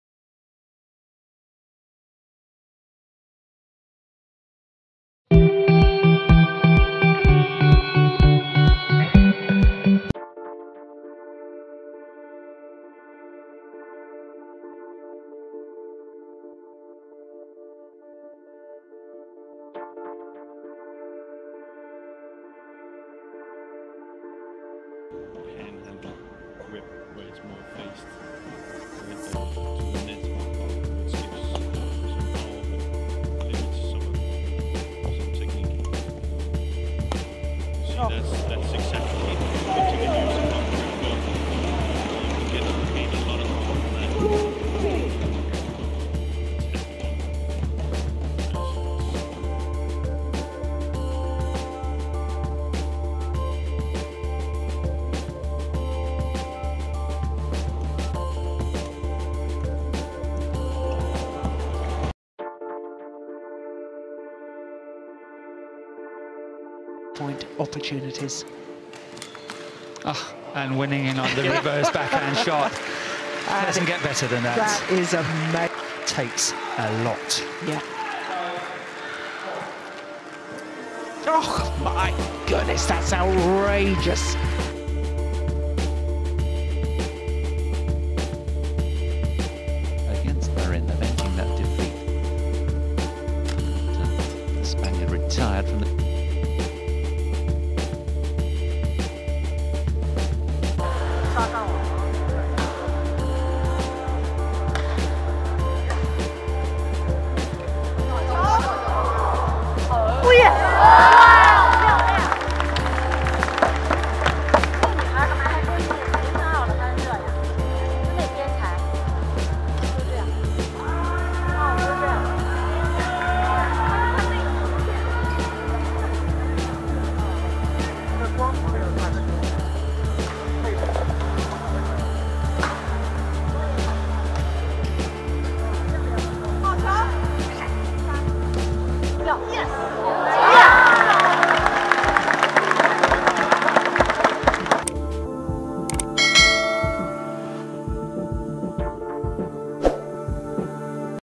TAI TZU YING(Deception) VS EXPERIENCED/RETIRED Players
This video is all about the deceptions of Tai tzu ying against players with lots of experience as well as Retired players.